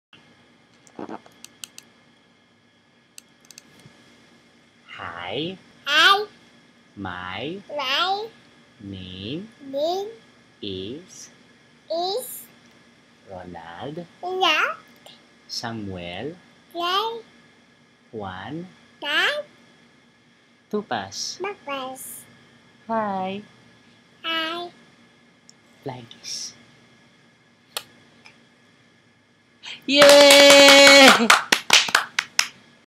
Tugz learning to introduce himself
"Hi! My name is Ronald Samuel Juan Tupas! Hi!" wave + flying kiss